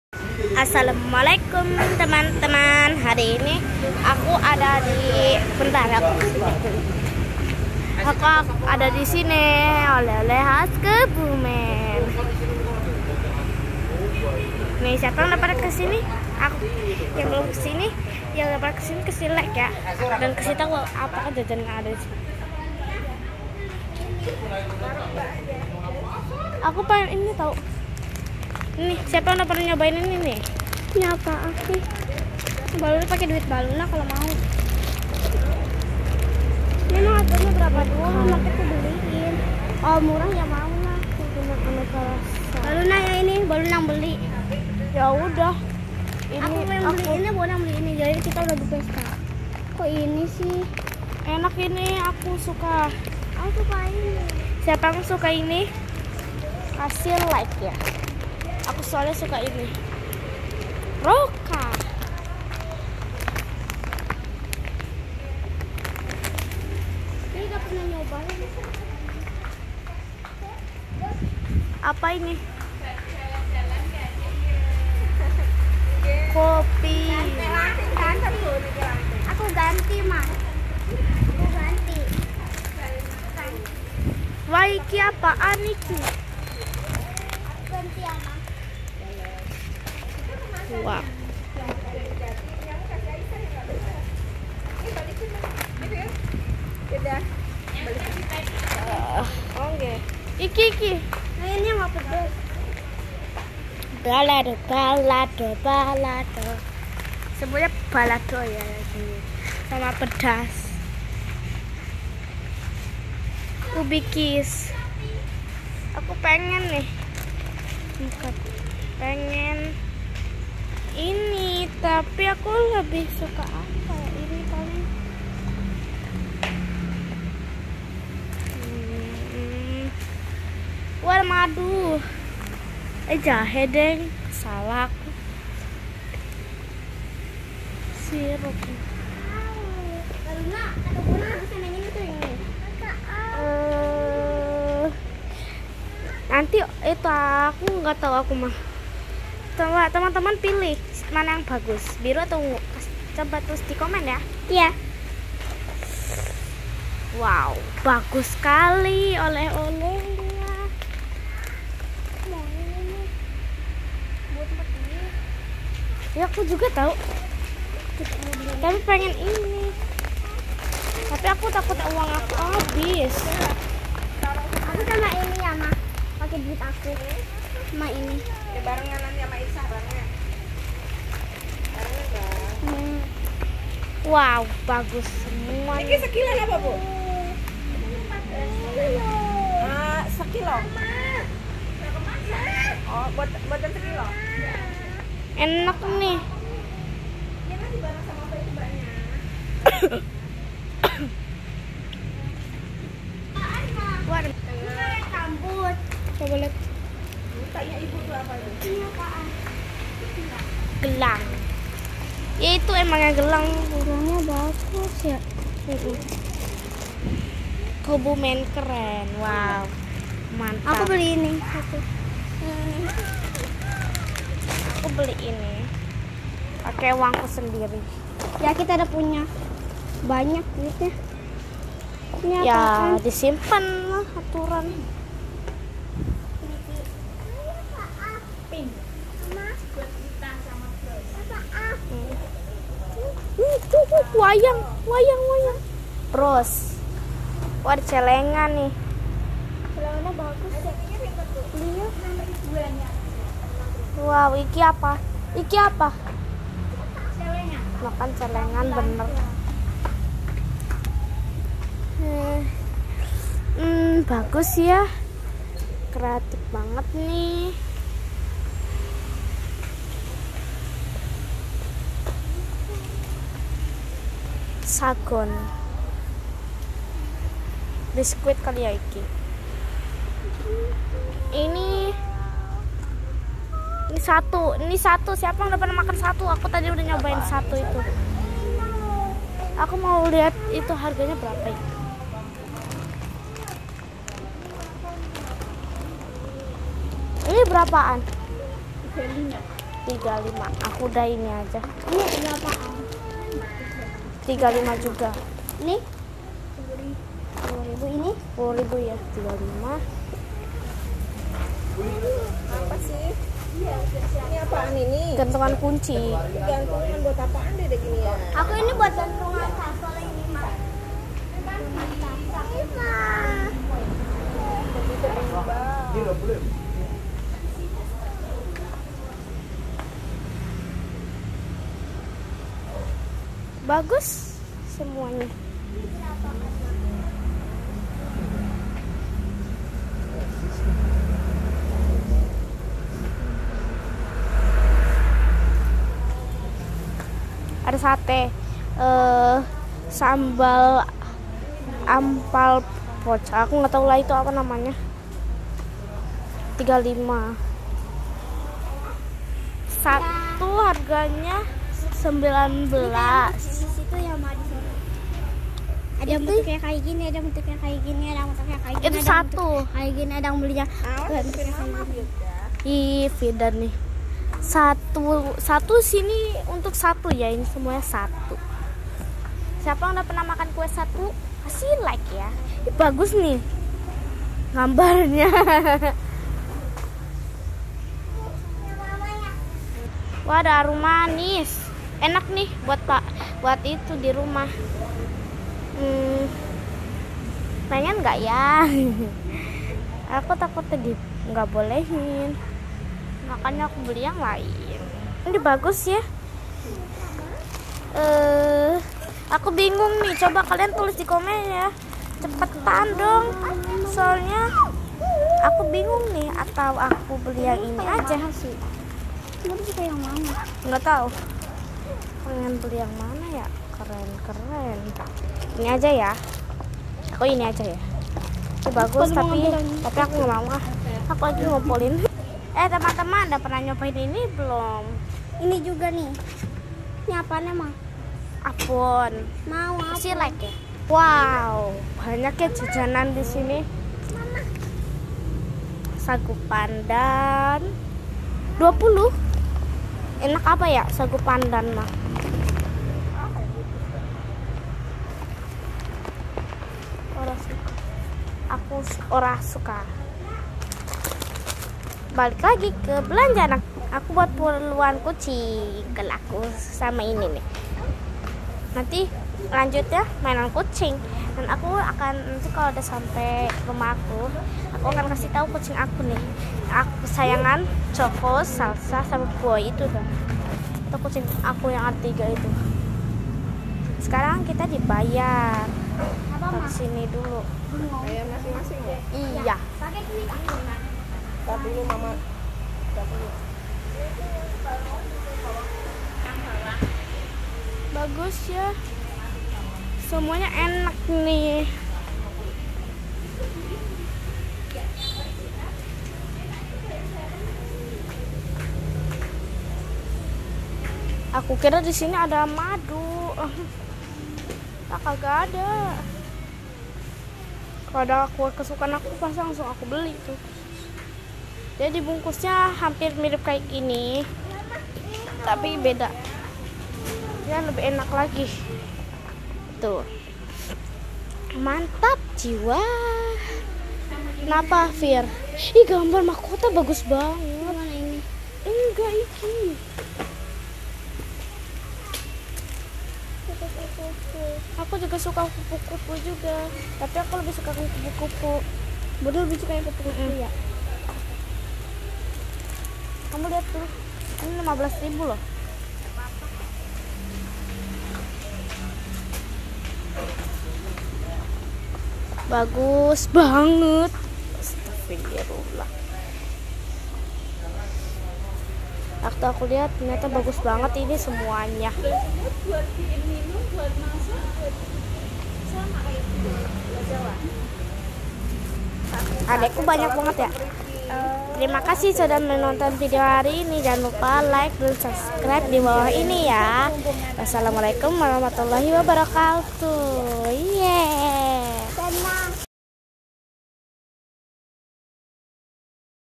Toko Mekar Pusat Oleh-Oleh Kota Kebumen
Toko Mekar adalah toko yang menjual oleh-oleh khas kota Kebumen.
Bagi yang ingin membeli aneka oleh-oleh khas kota Kebumen silahkan datang ke toko Mekar, lokasinya strategis berada di pusat kota Kebumen

Jl. Kusuma, Wonoyoso, Bumirejo, Kec. Kebumen, Kabupaten Kebumen, Jawa Tengah 54316